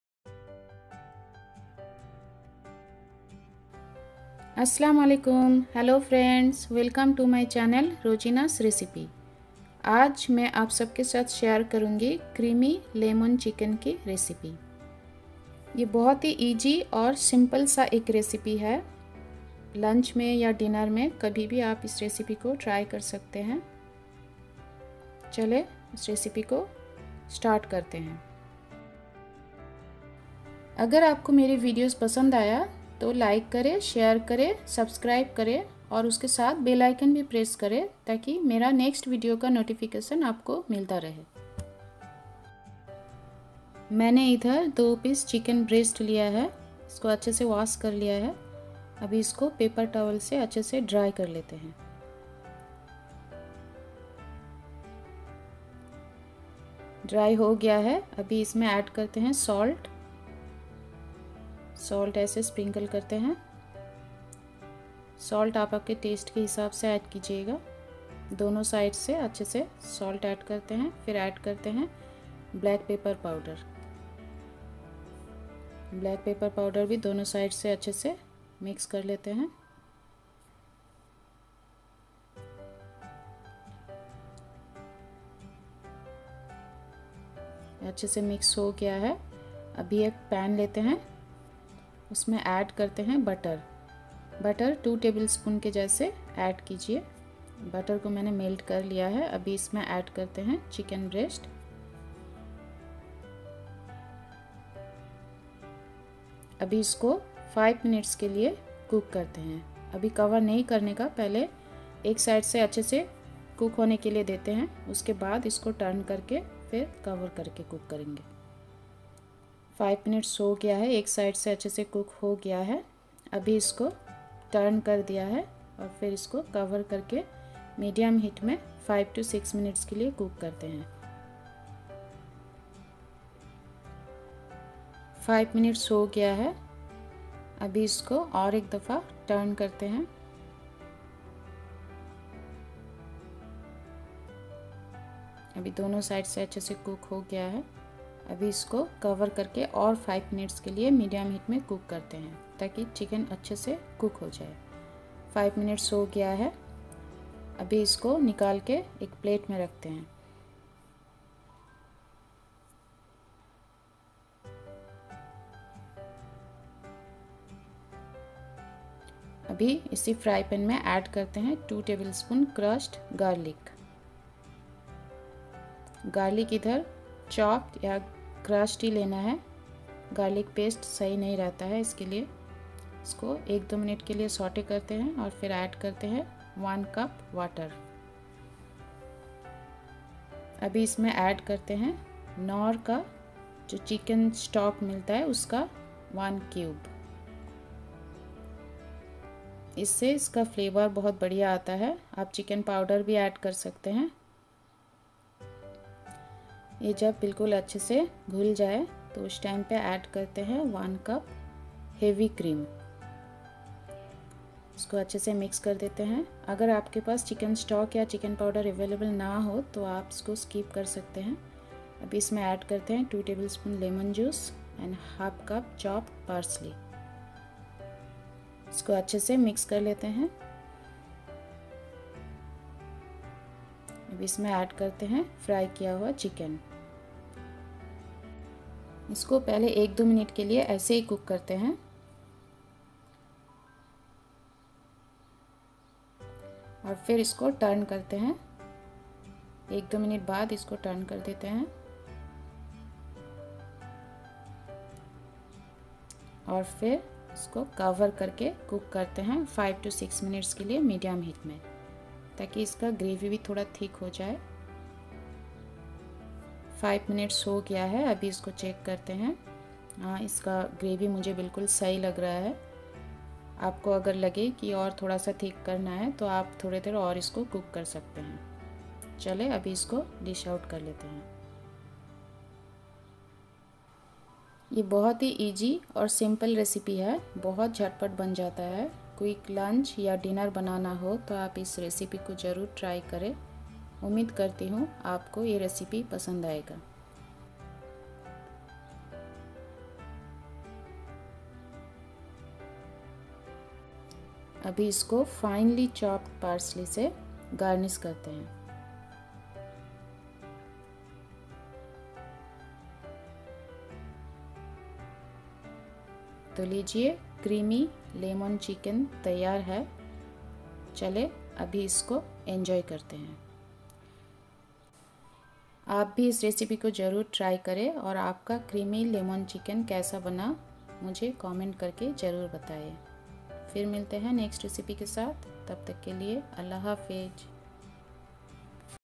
Creamy Garlic Chicken Breast Recipe
Chicken breast recipes | Keto chicken recipe | Creamy garlic chicken | How to make Creamy garlic chicken

Today I'm making an easy Creamy garlic chicken recipe. This recipe is extremely versatile and can be turned into creamy garlic chicken pasta, creamy garlic chicken and rice, creamy garlic chicken and mushrooms, the list goes on! This one pot chicken recipe is perfect for weeknight as well as a meal prep option. You can also switch the chicken breast for chicken thighs or any other part.  Give this a shot and it's surely going to turn into your favourite quick dinner recipe!

INGREDIENTS
2 boneless chicken breasts
Salt
Black pepper
2 tbsp butter
4 cloves garlic
1 cup chicken stock
1 cup heavy cream
2 tbsp lemon juice
1/2 cup chopped persely